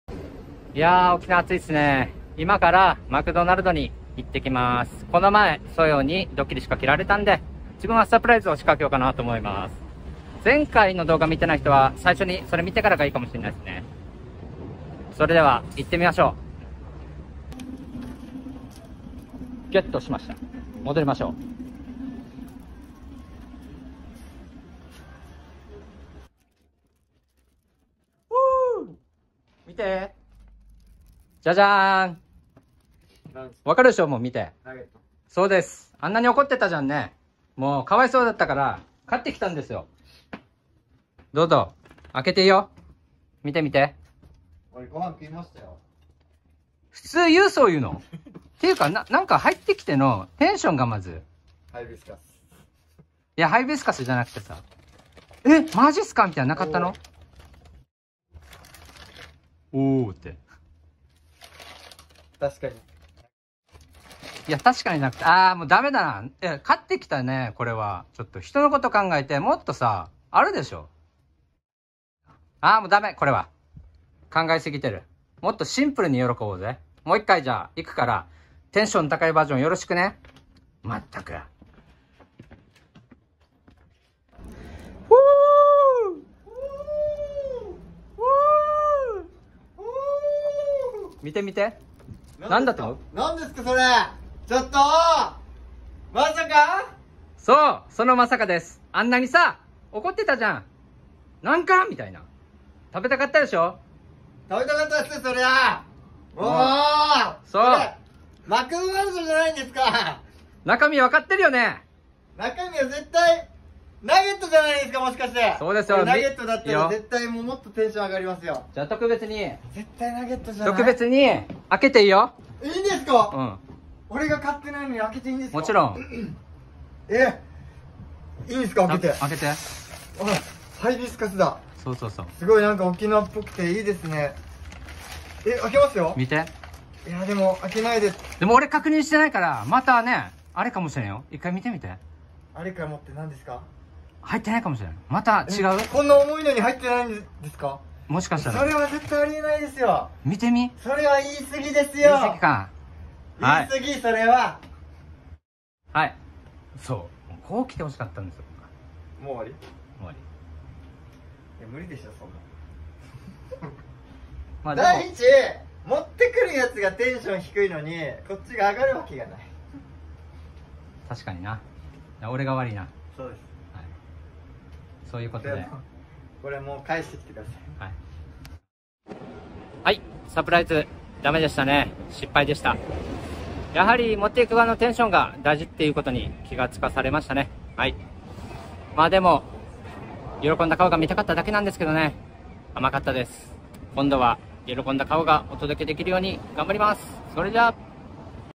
急にマクドナルドを差し入れしたら反応悪過ぎと撮り直した回
サプライズ　マクドナルド

LEFLAH沖縄店
沖縄県那覇市牧志2-7-24 1F leflah沖縄店

モノレール牧志駅から徒歩30秒から5分